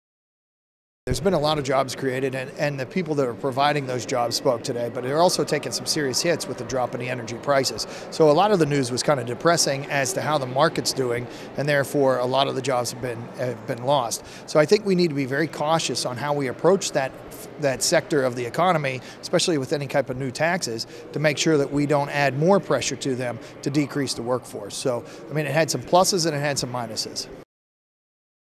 A Cautious Approach Toward Natural Gas
PA State Rep Ted Nesbit explains that a cautious approach must be used toward the Natural Gas Industry and that care must be taken when creating taxation.